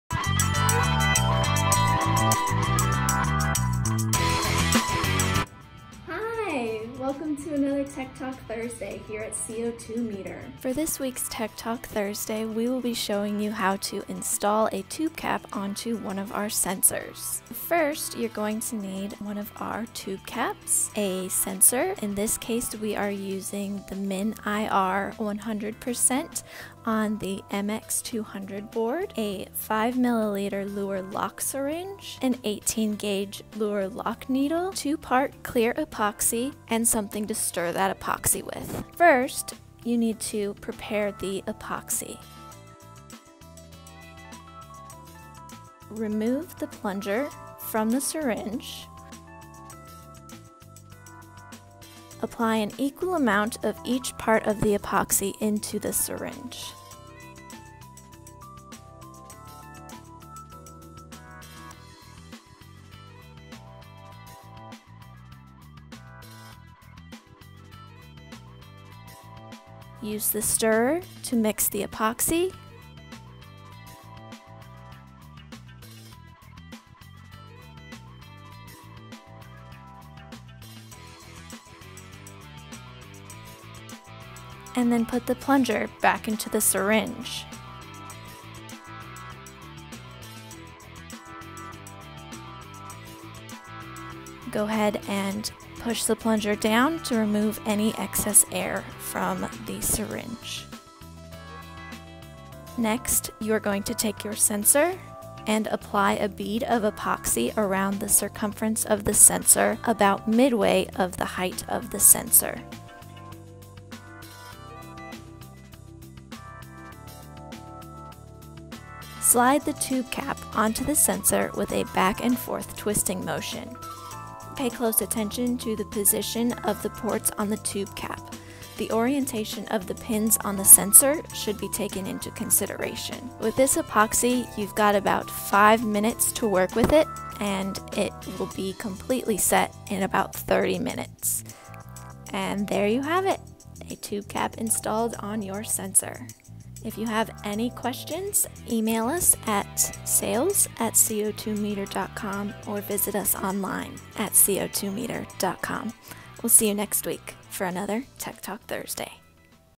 How To Install a Tube Cap on Our Sensors
For this week’s Tech Talk Thursday we are doing a how-to video! Kara will be showing you how to install one of our new Tube Caps onto our sensors.

Tube caps are great accessories for a wide range of our sensors – they easily convert them from diffusion applications to sampling applications.